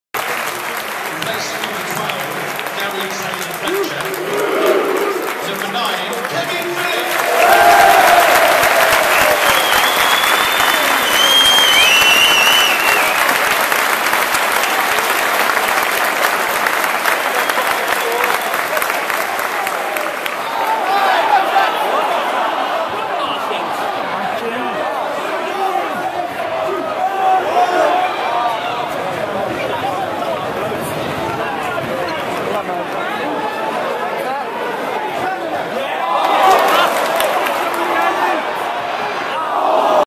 Kevin Phillips comes on for Blackpool V Birmingham 31.12.2011
Recorded on December 31, 2011 using a Flip Video camera. Kevin Phillips comes on for Blackpool against Birmingham City where he is given applause. A blues great who did great things for us. Was personally gutted when he left but he did, and he is now  doing well for Blackpool. KRO.